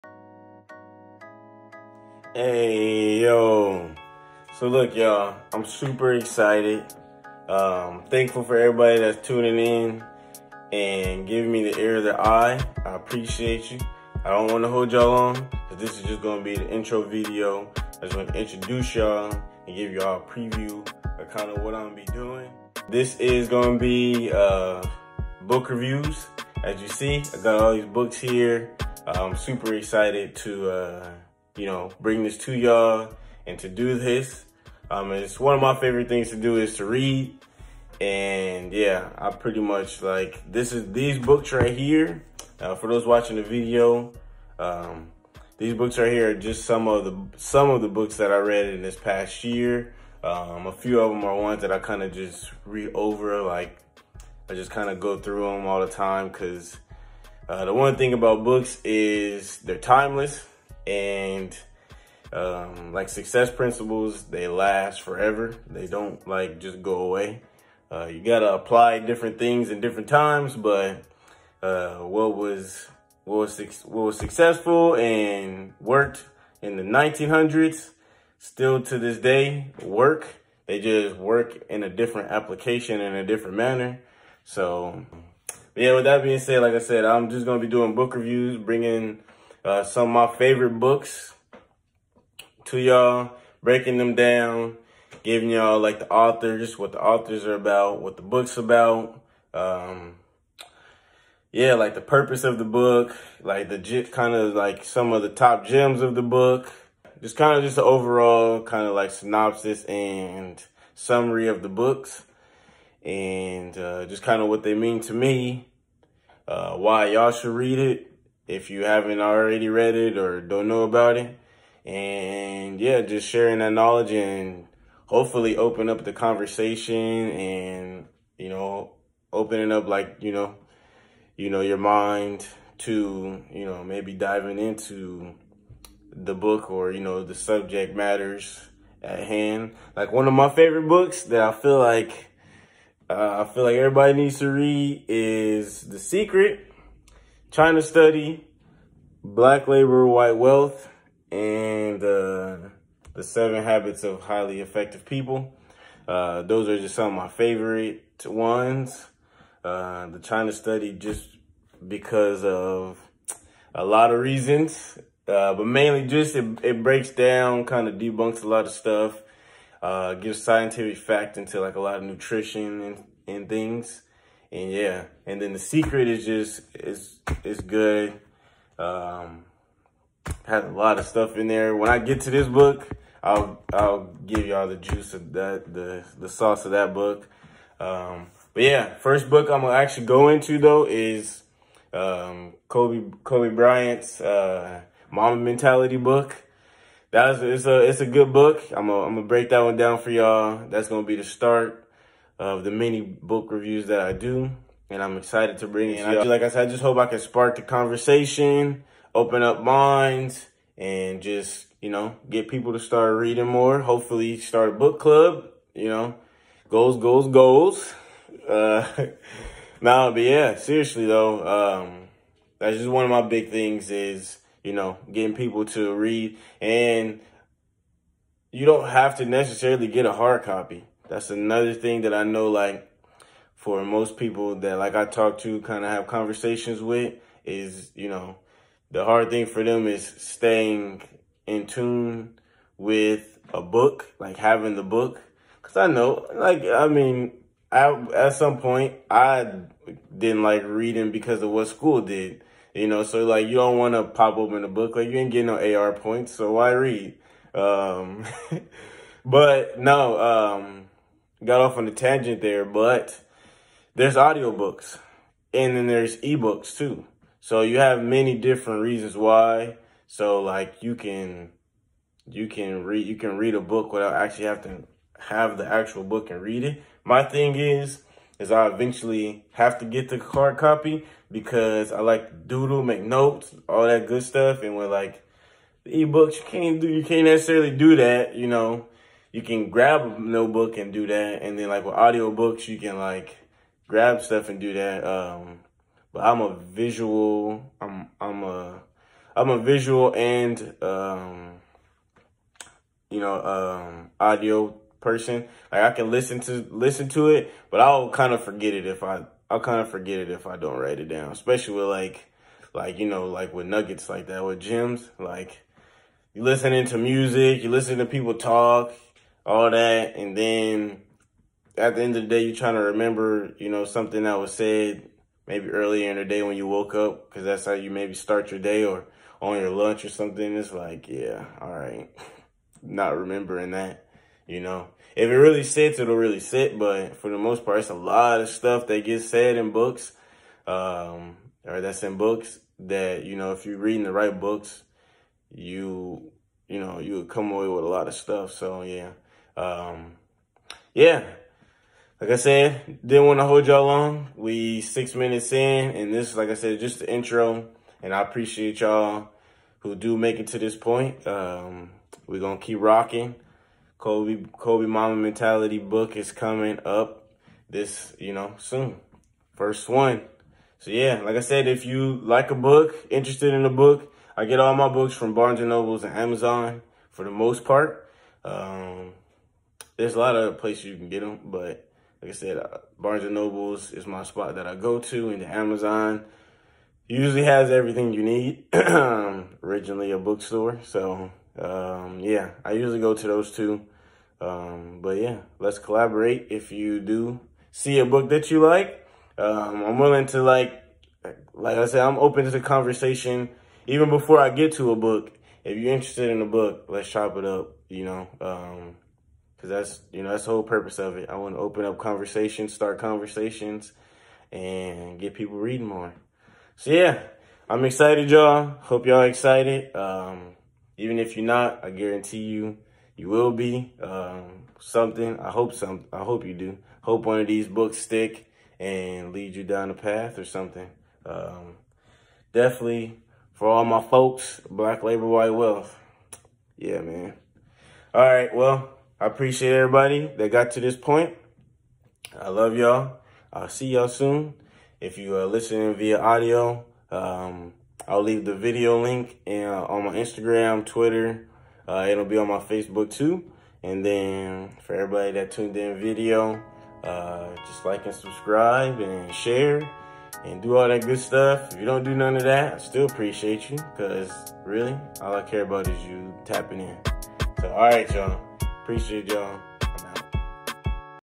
Introduction To The Book Review Show
This is the introduction into what the Book Review show will consist of and what to expect along with why this all means so much to me.